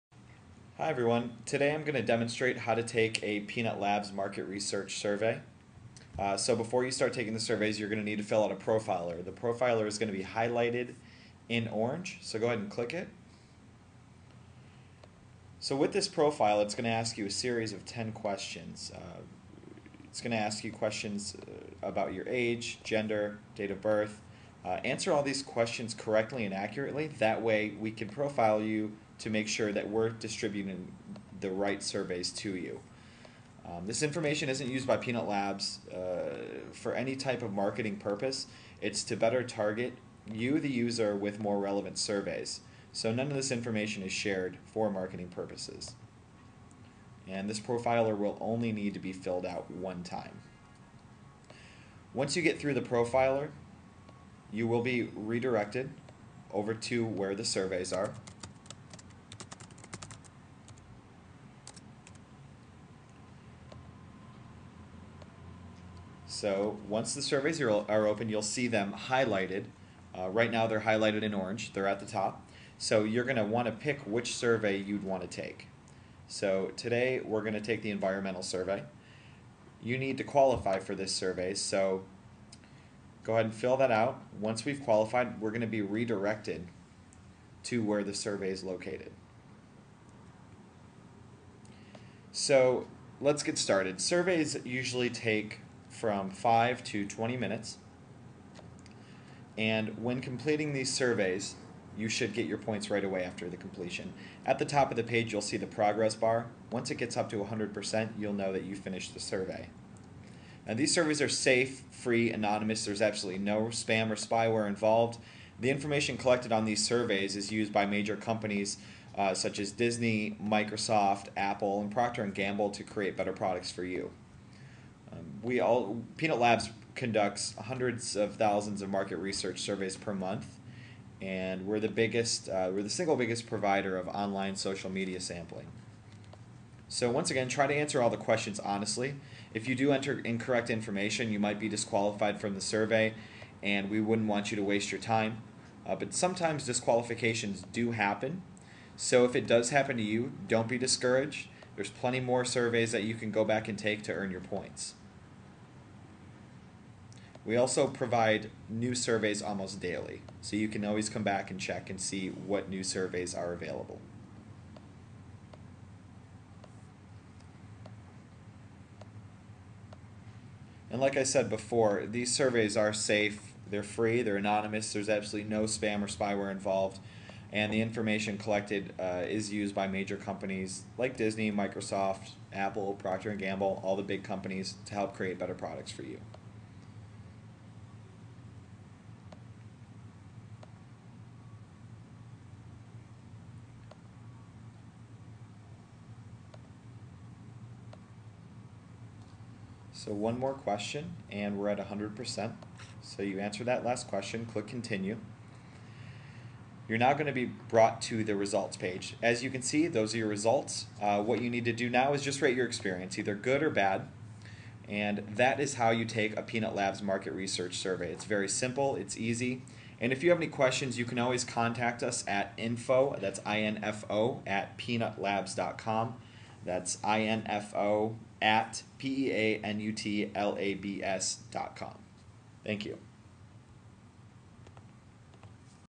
Peanut Labs Survey Tutorial
This video is a tutorial that will show a user how to navigate a Peanut Labs market research survey